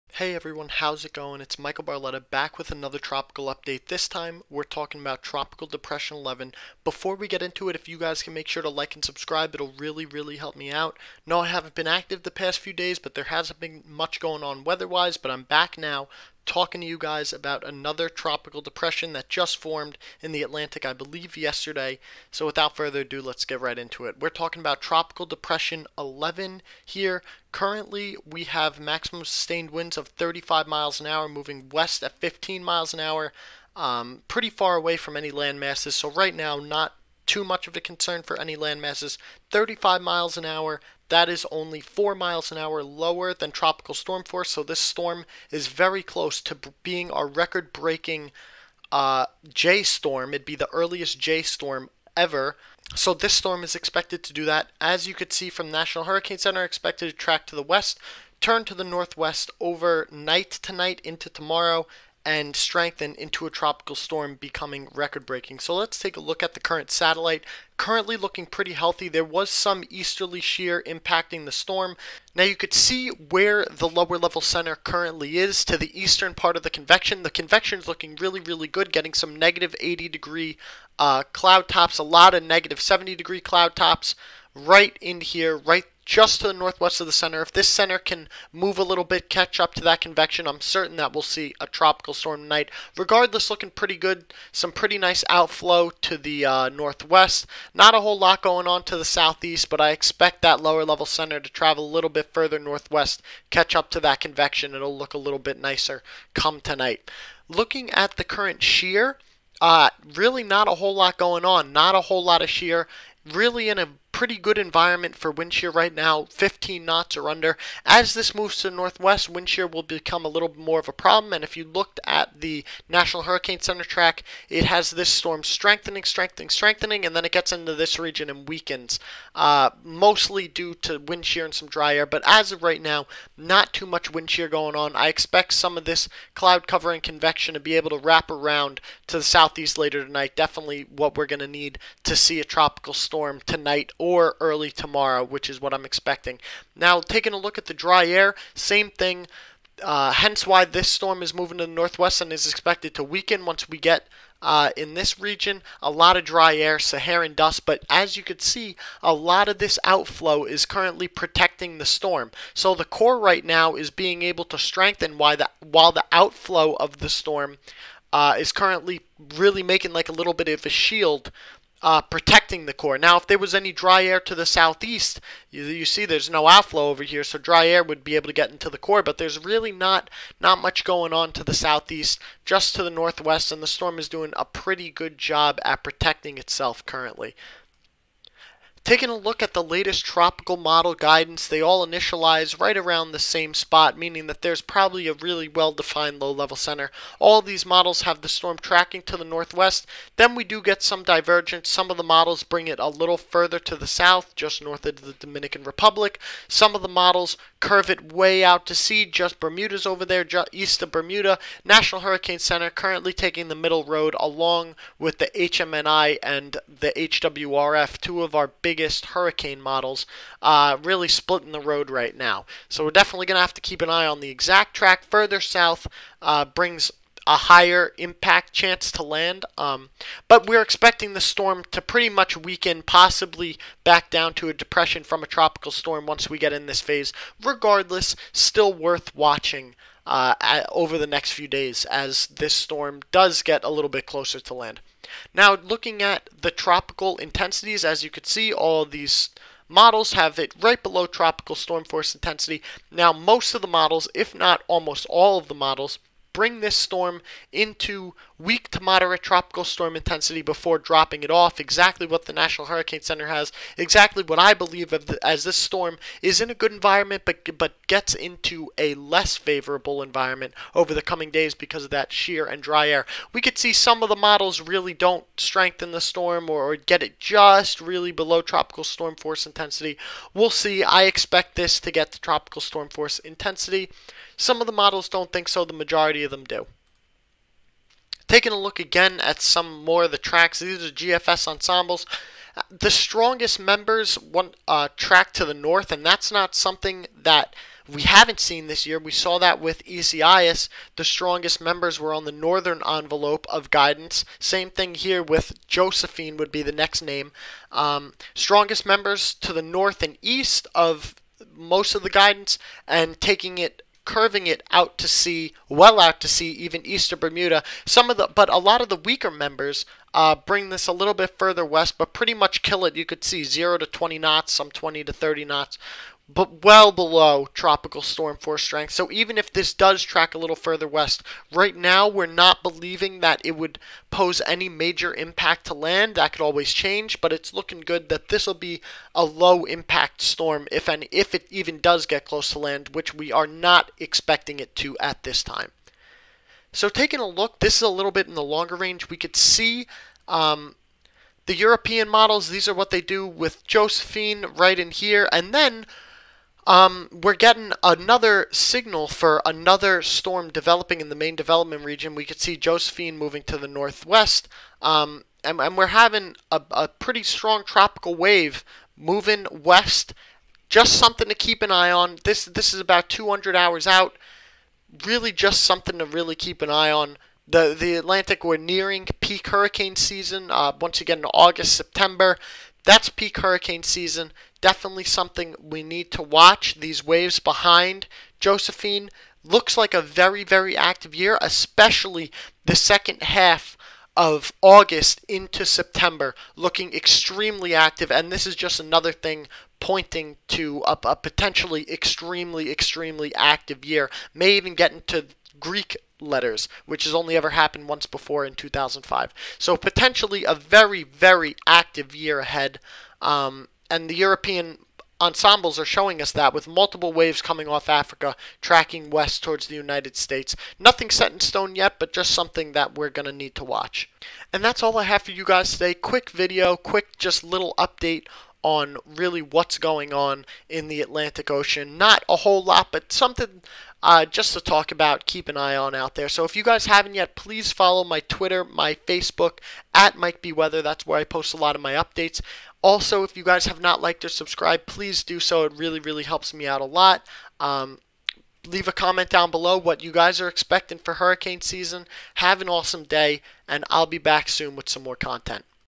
Record Breaking 10th Tropical Storm Expected in Atlantic
A record breaking 10th tropical storm is expected to form in the Atlantic this week it would be named Josephine. An active period is expected soon in the Atlantic.